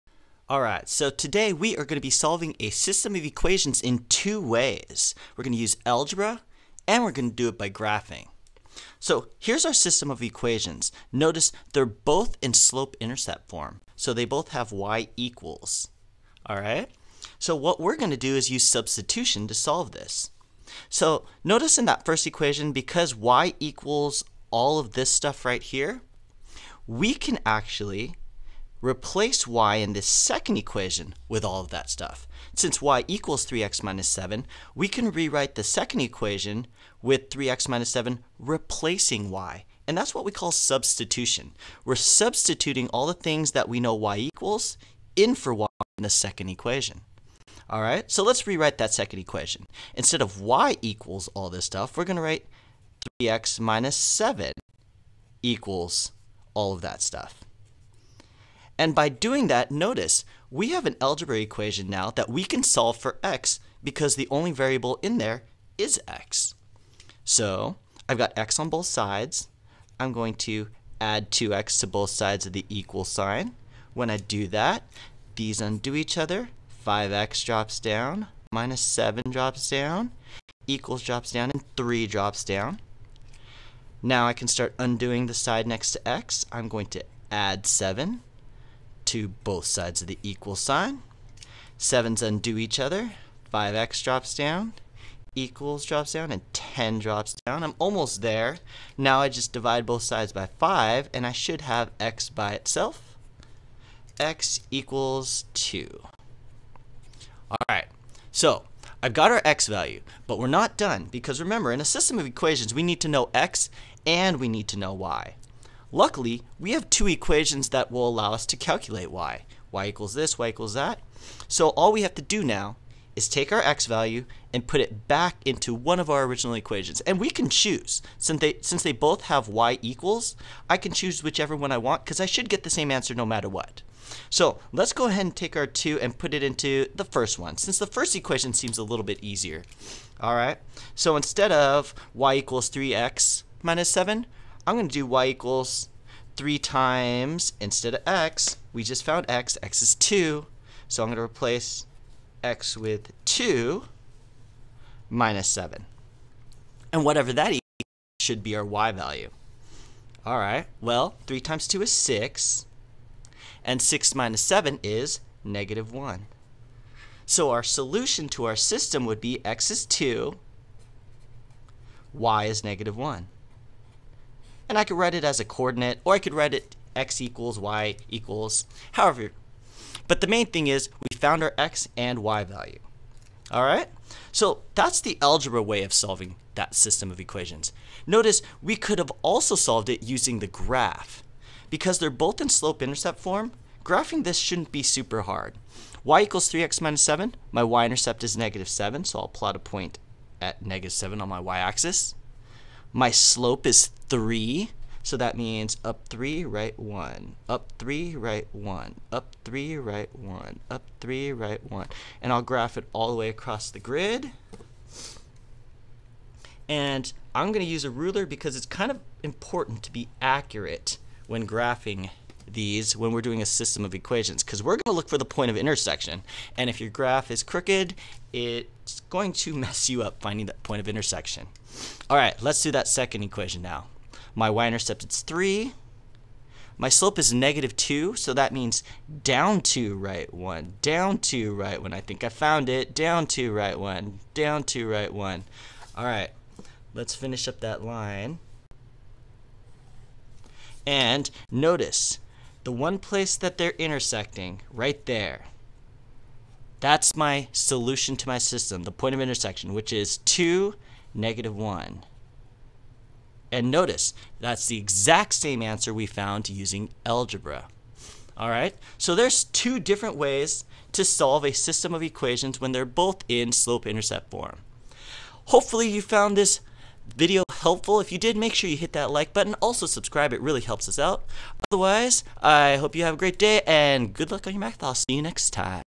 Solving a System of Equations (both y=)
Solving systems of equations can be tricky!  In this video, I show you how to solve a system of equations when they are both in slope-intercept form.  I do it in two ways - using algebra (substitution) and by graphing!